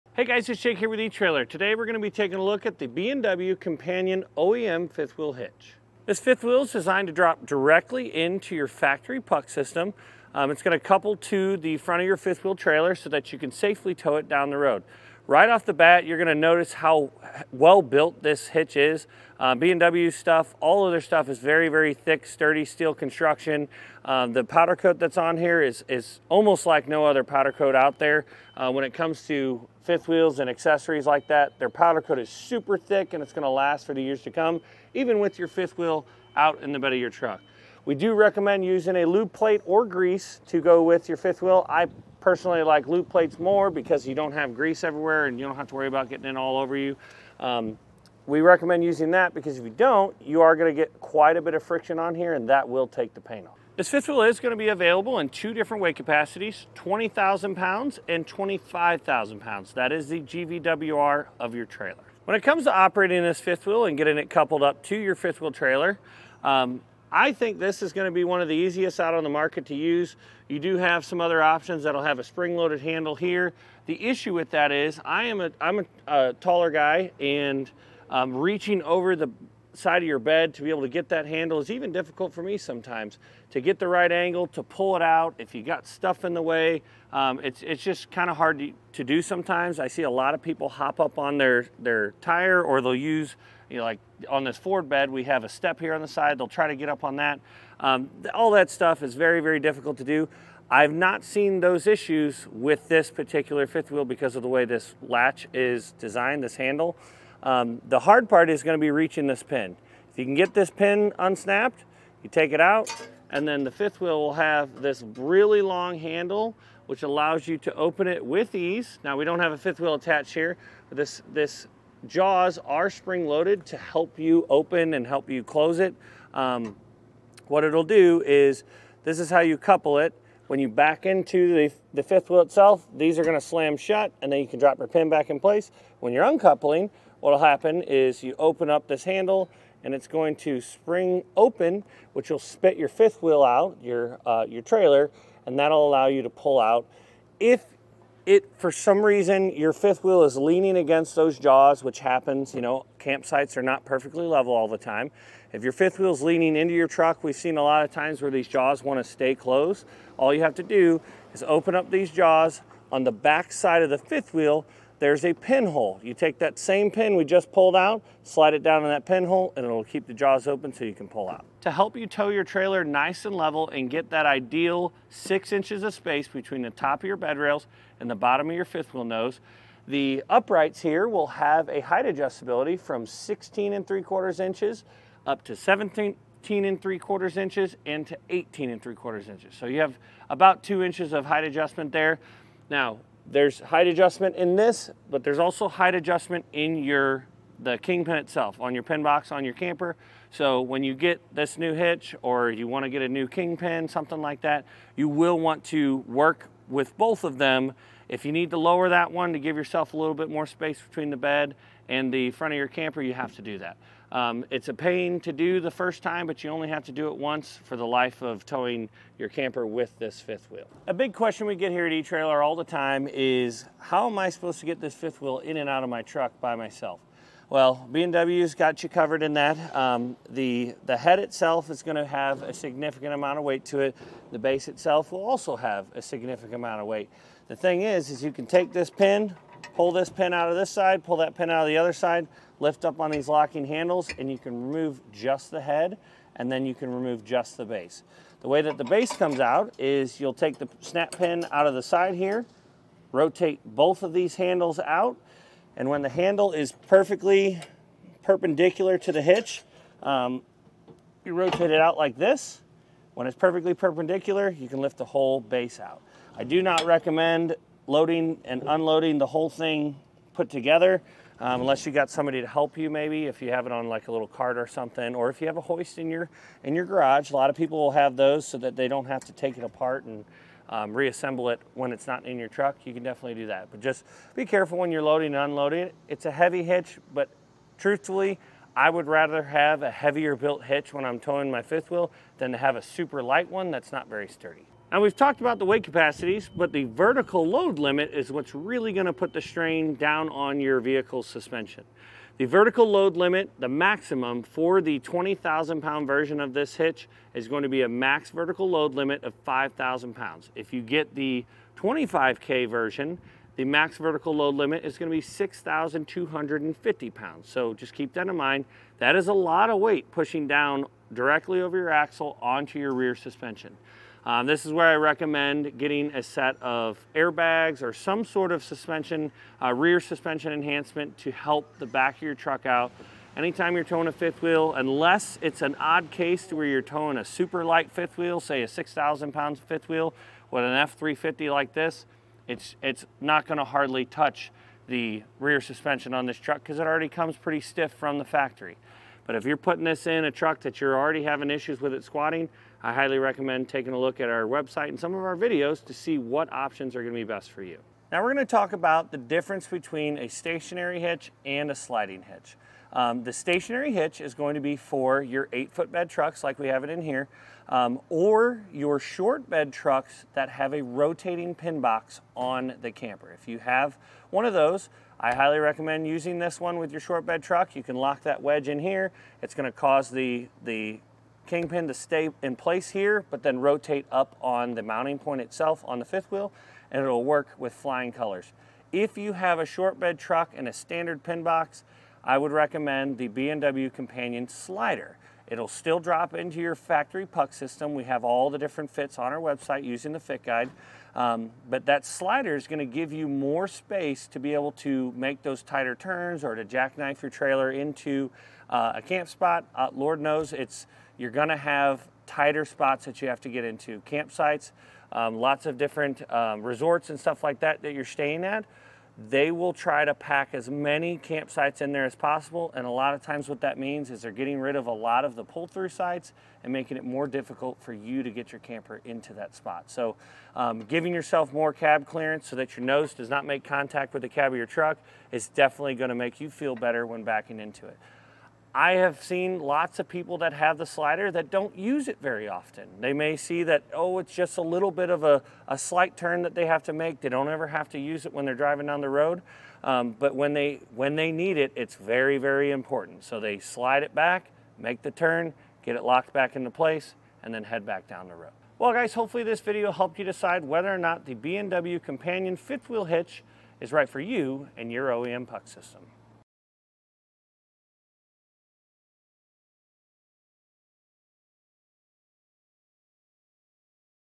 etrailer | B and W Companion OEM 5th-Wheel Hitch for Ford Super Duty Prep Package: The Breakdown
Click for more info and reviews of this B and W 5th Wheel Hitch:

Hey guys, it's Jake here with etrailer.  Today we're gonna be taking a look  at the B&W Companion OEM Fifth Wheel Hitch.  This fifth wheel is designed  to drop directly into your factory puck system.  It's gonna couple to the front of your fifth wheel trailer  so that you can safely tow it down the road.  Right off the bat, you're gonna notice  how well-built this hitch is.  B&W stuff, all of their stuff is very, very thick,  sturdy steel construction.

The powder coat that's on here is almost like  no other powder coat out there when it comes  to fifth wheels and accessories like that.  Their powder coat is super thick,  and it's gonna last for the years to come,  even with your fifth wheel out in the bed of your truck.  We do recommend using a lube plate  or grease to go with your fifth wheel.  I personally like lube plates more  because you don't have grease everywhere,  and you don't have to worry about getting it all over you.  We recommend using that because if you don't,  you are gonna get quite a bit of friction on here,  and that will take the paint off.

This fifth wheel is gonna be available  in two different weight capacities:  20,000 pounds and 25,000 pounds.  That is the GVWR of your trailer.  When it comes to operating this fifth wheel  and getting it coupled up to your fifth wheel trailer,  I think this is gonna be one  of the easiest out on the market to use.  You do have some other options  that'll have a spring loaded handle here.  The issue with that is I'm a taller guy,  and reaching over the side of your bed  to be able to get that handle  is even difficult for me sometimes.

To get the right angle, to pull it out,  if you got stuff in the way, it's just hard to do sometimes.  I see a lot of people hop up on their tire,  or they'll use, like on this Ford bed,  we have a step here on the side,  they'll try to get up on that.  All that stuff is very, very difficult to do.  I've not seen those issues with this particular fifth wheel  because of the way this latch is designed, this handle.  The hard part is gonna be reaching this pin.

If you can get this pin unsnapped, you take it out,  and then the fifth wheel will have this really long handle,  which allows you to open it with ease.  Now we don't have a fifth wheel attached here,  but this jaws are spring loaded to help you open  and help you close it.  What it'll do is this is how you couple it.  When you back into the fifth wheel itself,  these are gonna slam shut  and then you can drop your pin back in place.  When you're uncoupling,  what'll happen is you open up this handle,  and it's going to spring open,  which will spit your fifth wheel out, your trailer,  and that'll allow you to pull out.  If for some reason your fifth wheel  is leaning against those jaws, which happens,  campsites are not perfectly level all the time,  if your fifth wheel's leaning into your truck,  we've seen a lot of times  where these jaws wanna stay closed.  All you have to do is open up these jaws.  On the backside of the fifth wheel, there's a pinhole.  You take that same pin we just pulled out,  slide it down in that pinhole,  and it'll keep the jaws open so you can pull out.  To help you tow your trailer nice and level  and get that ideal six inches of space  between the top of your bed rails  and the bottom of your fifth wheel nose,  the uprights here will have a height adjustability  from 16 3/4 inches up to 17 3/4 inches and to 18 3/4 inches.  So you have about two inches of height adjustment there.  Now there's height adjustment in this,  but there's also height adjustment  in the kingpin itself on your pin box on your camper.  So when you get this new hitch,  or you want to get a new kingpin, something like that,  you will want to work with both of them.  If you need to lower that one  to give yourself a little bit more space between the bed  and the front of your camper, you have to do that.  It's a pain to do the first time,  but you only have to do it once  for the life of towing your camper with this fifth wheel.  A big question we get here at etrailer all the time is,  "How am I supposed to get this fifth wheel  in and out of my truck by myself"  Well B&W's got you covered in that.  The head itself is gonna have  a significant amount of weight to it.  The base itself will also have  a significant amount of weight.  The thing is, is you can take this pin,  pull this pin out of this side,  pull that pin out of the other side,  lift up on these locking handles,  and you can remove just the head  and then you can remove